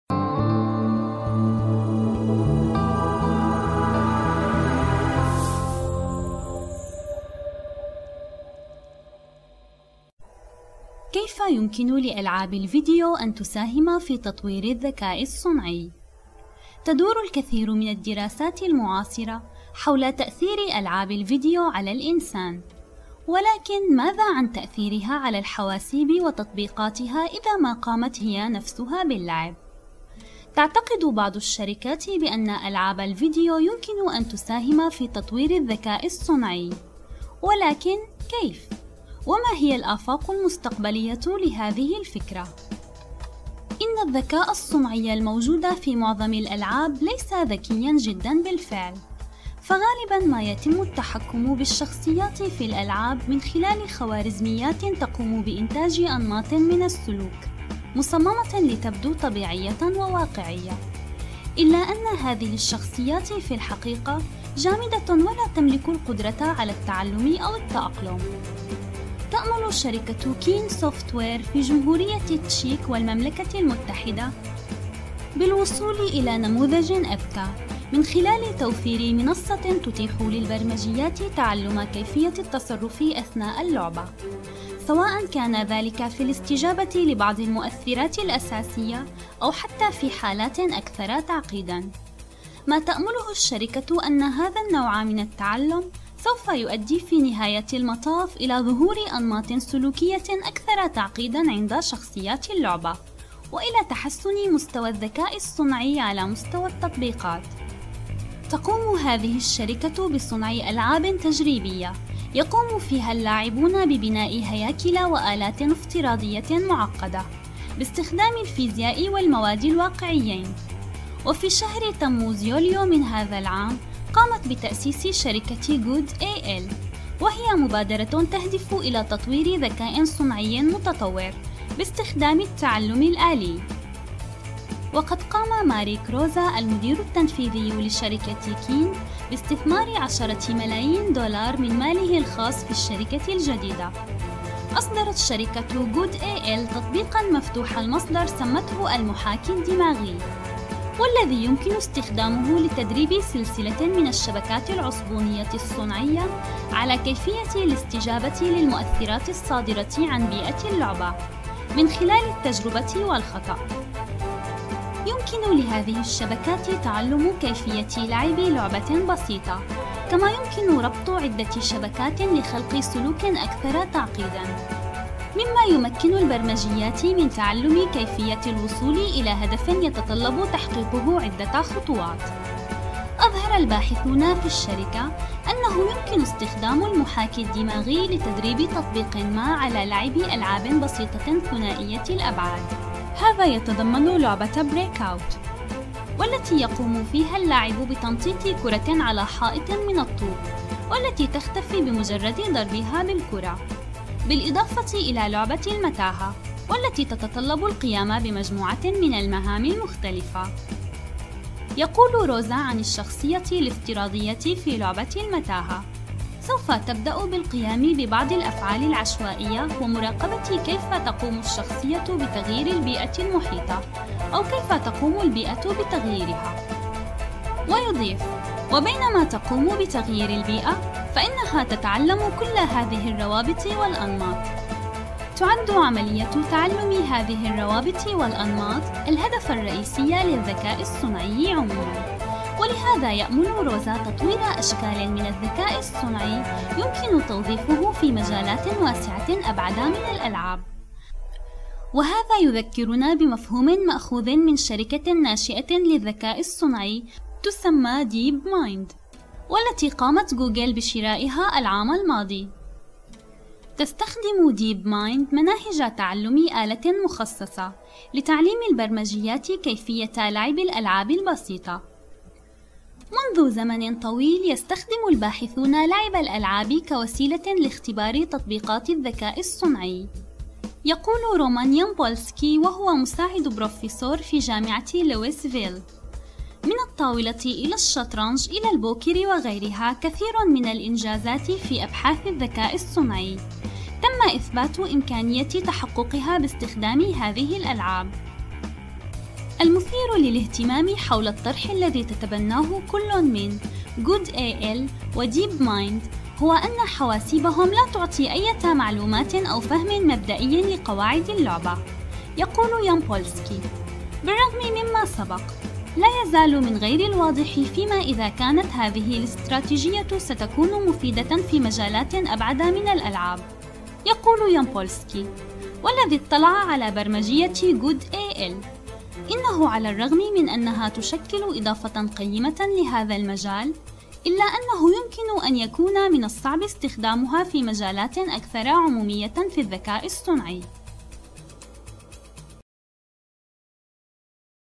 كيف يمكن لألعاب الفيديو أن تساهم في تطوير الذكاء الصنعي؟
صوت: Soha A Kareem